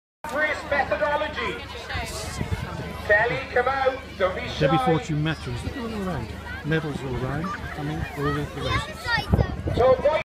Chew Stoke Harvest Home Kids Races
The kid's races from Chew Stoke Harvest Home taken by John Tiley.